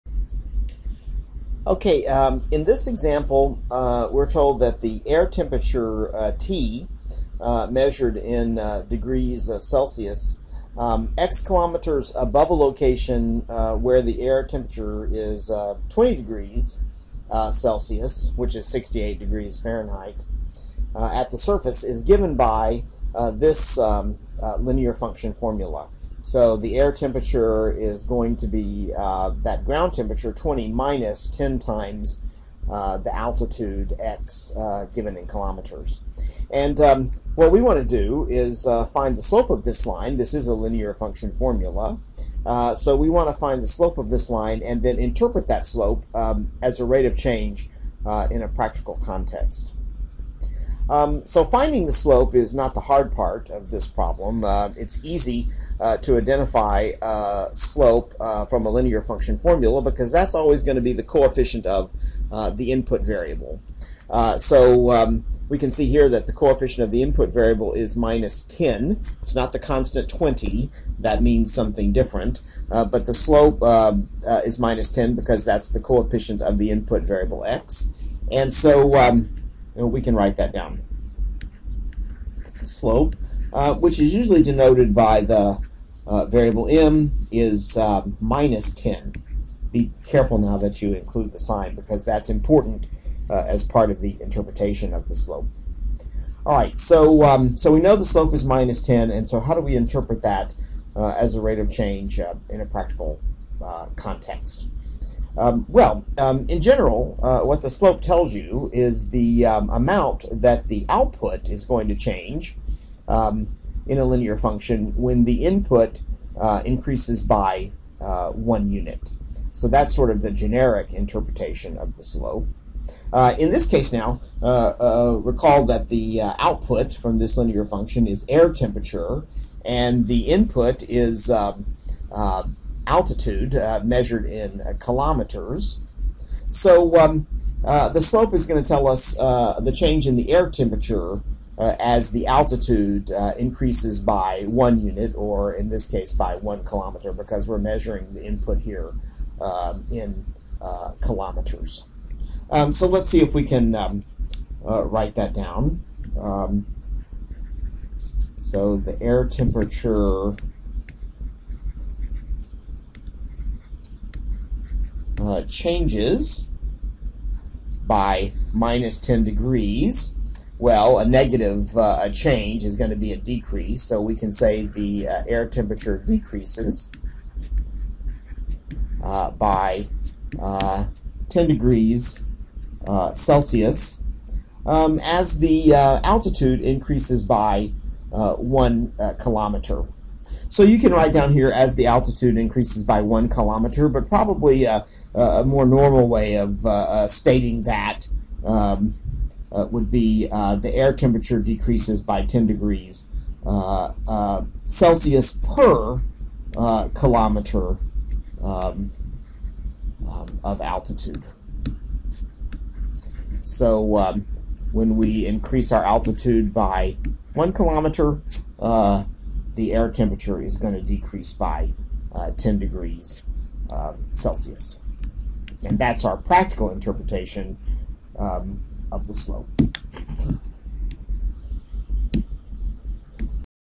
Interpreting slope in context
How to interpret the slope of a line in a practical context